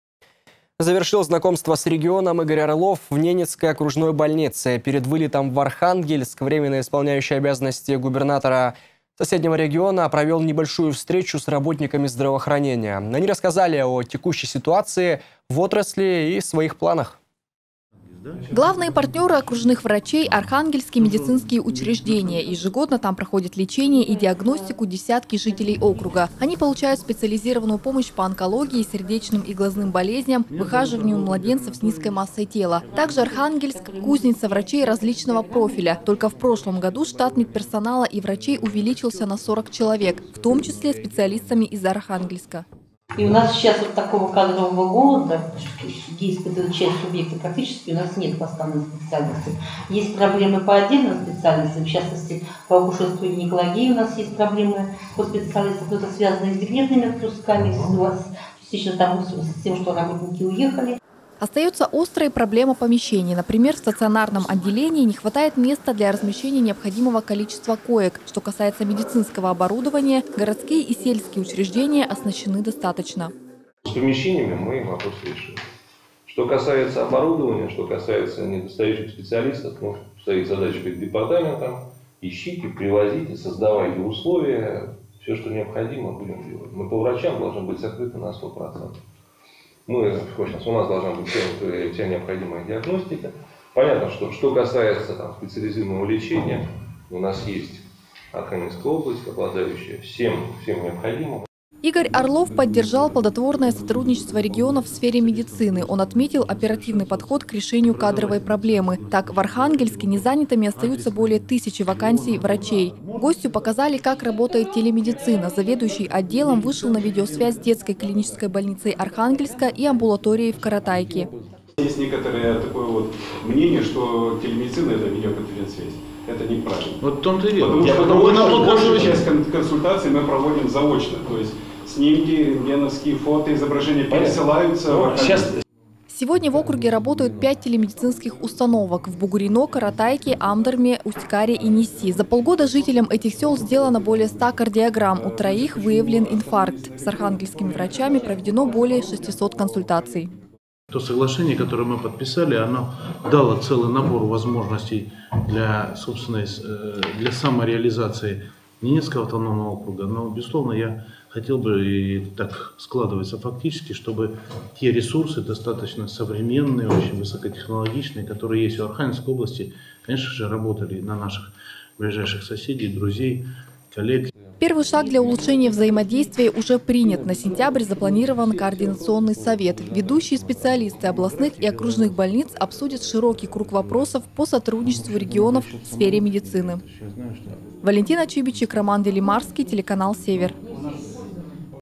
Главные партнеры окружных врачей НАО — медицинские учреждения из Архангельска
Делегация из Архангельска во главе с Игорем Орловым завершили знакомство с регионом в Ненецкой окружной больнице, где прошла небольшая встреча с работниками здравоохранения. Они рассказали о текущей ситуации в отрасли и своих планах.

Главные партнеры окружных врачей — Архангельские медицинские учреждения. Ежегодно там проходят лечение и диагностику десятки жителей округа. Они получают специализированную помощь по онкологии, сердечным и глазным болезням, выхаживанию младенцев с низкой массой тела. Также Архангельск — кузница врачей различного профиля. Только в прошлом году штат медперсонала и врачей увеличился 40 человек, в том числе специалистами из Архангельска.